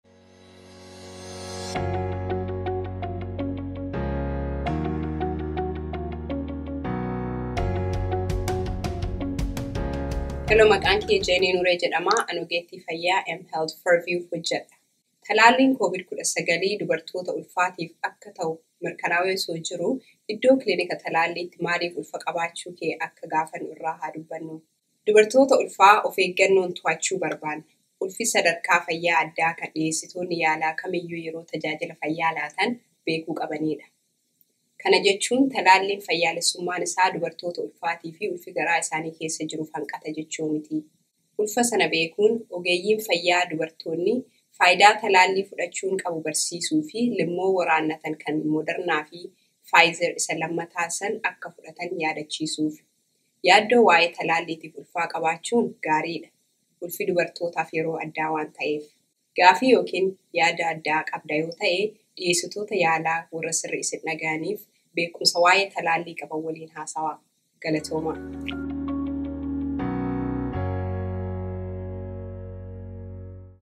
Oromo Vaccine Pregnancy Jenny Video 3
If the vaccine is okay for pregnancy, why am I asked if I’m pregnant at a vaccine clinic?

Yoo talaalliin kun dubartoota ulfaatiif kan tahu tahe ulfa qabaachuu koo iddoo kilinika talaalliitti maaliifiin gaafatama?